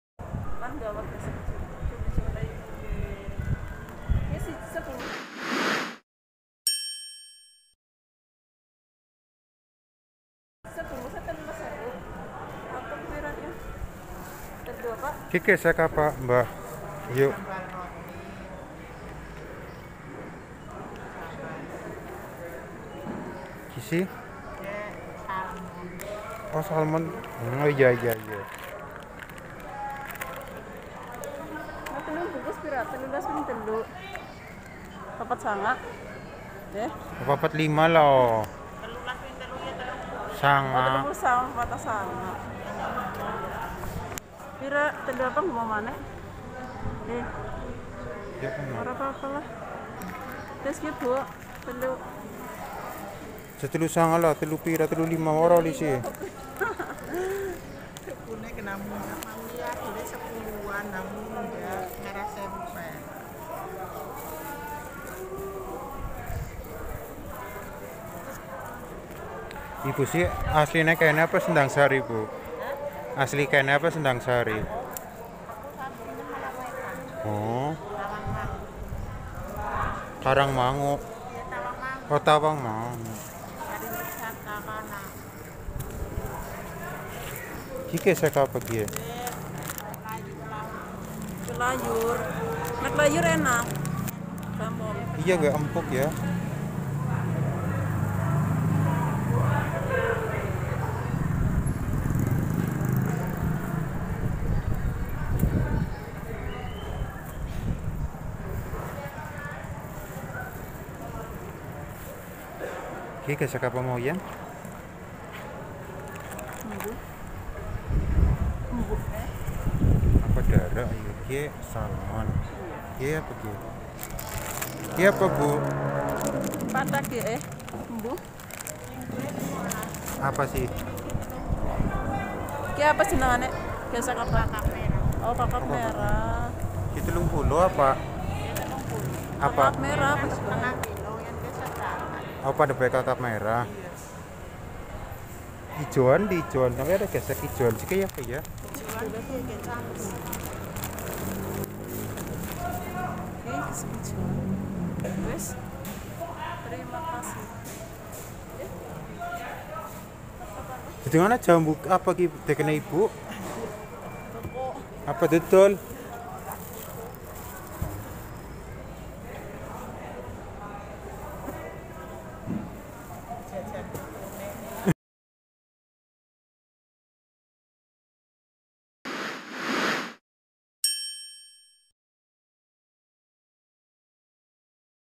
Ikan Asin Cilacap | Belanja Ikan Asin Di Tempat Pelelangan Ikan
Banyak sekali ikan asin dari berbagai macam ikan, ada dari ikan salmon, ikan kakap, ikan layur,  dan masih banyak sekali jenis ikan yang bisa dibuat ikan asin.